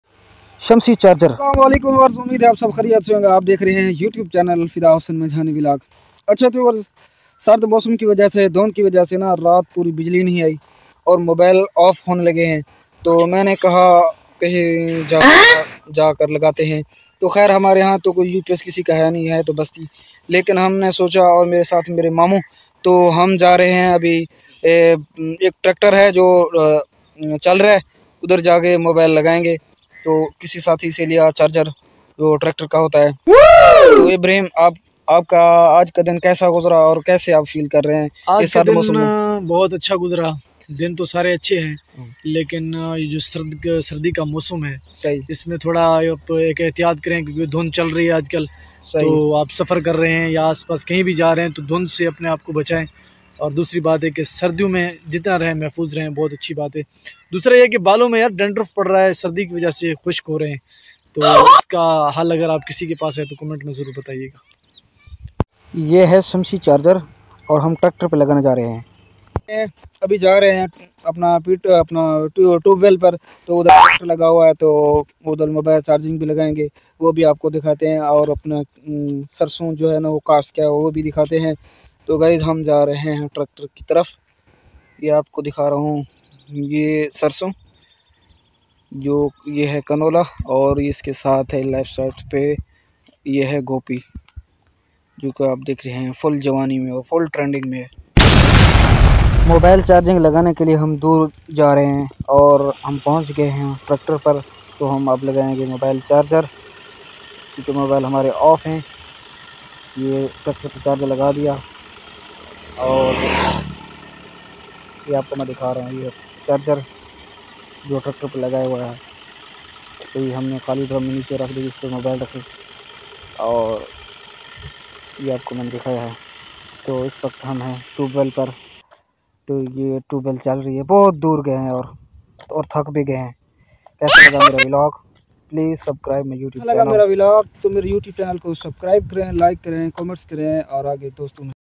Tractor par mobile charging Karne ka tarika ll bijali ki problem leukaemia vlog
tractor par mobile charging Karne ka tarika ll bijali ki problem leukaemia vlog youtube video vlog bijali na hone ki vajah se tractor per mobile charging karne ke liye dur Gaye khatu ma gaye.